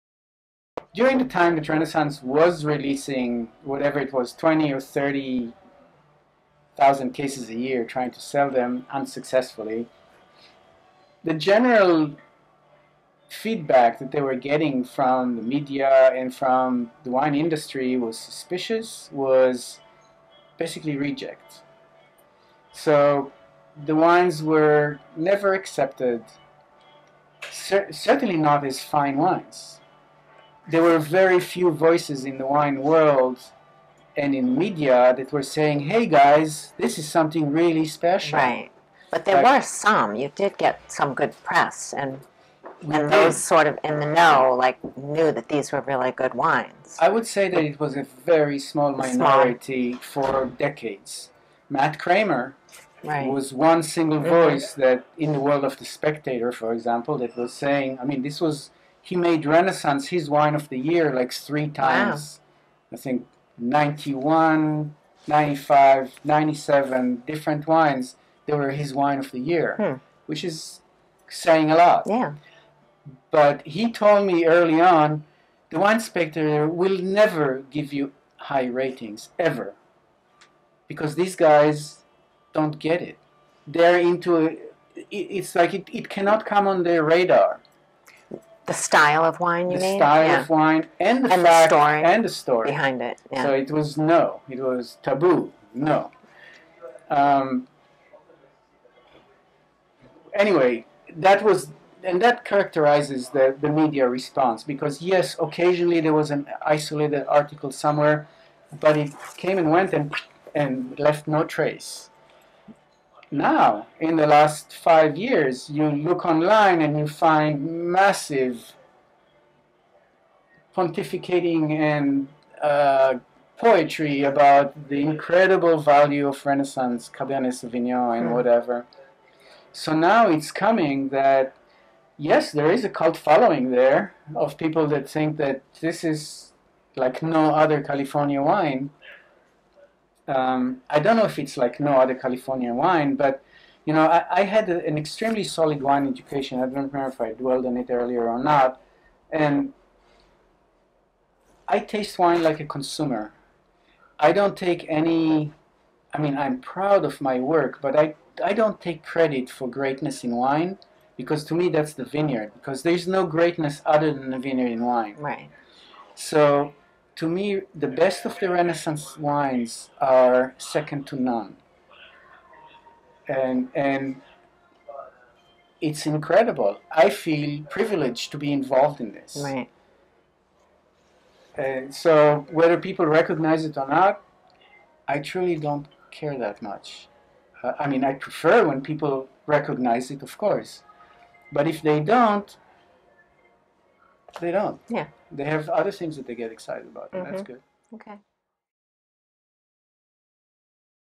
Gideon Beinstock of Clos Saron winery
Gideon Beinstock of Clos Saron winery in California's Sierra Foothills talks with Lisa Denning of Grape Collective about the critical response to the Renaissance Winery. The Renaissance Winery was founded by the Fellowship of Friends, which many consider a cult yet is being re-evaluated as having produced some of California’s finest wines.Videos about winemakers and wine.